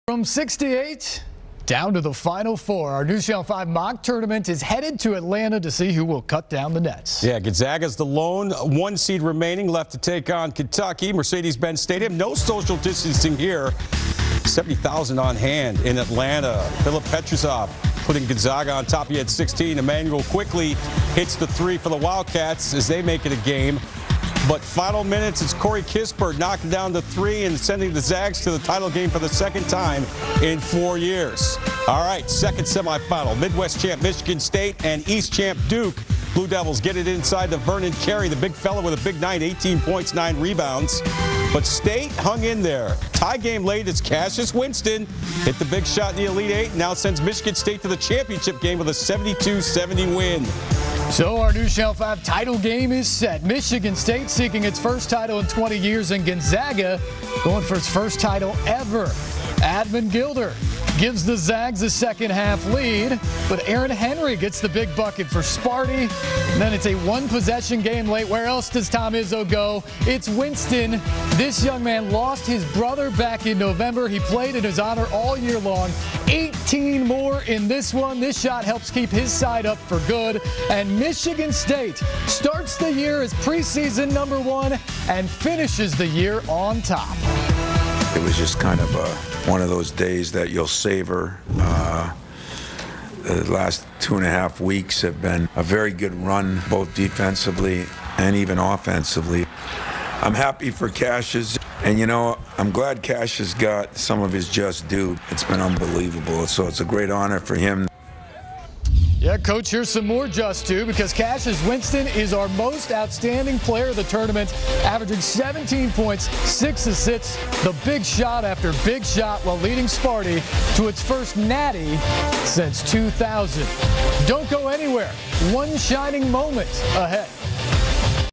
We simulate the NCAA Tournament p4
With all sports canceled due to the Coronavirus pandemic, Steve and Jon ask the question, what would have happened if the tournament went down as planned. Tune in to find out the results.